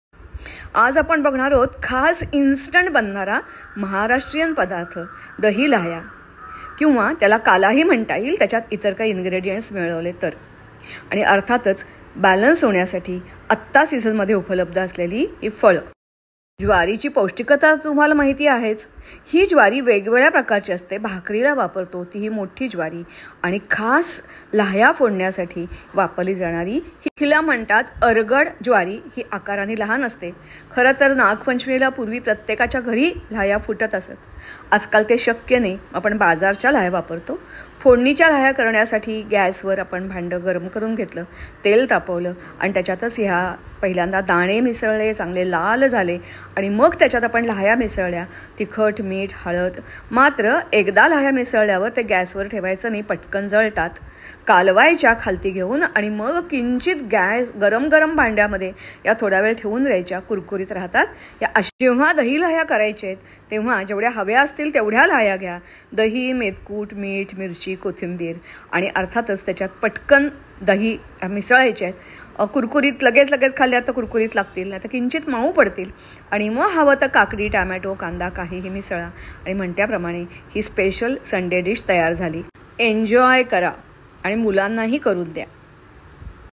झटपट होणारा ☀ day special. दही - लाहया
Anybody can make it in 1minute. just mix it,easy to digest and healthy to eat.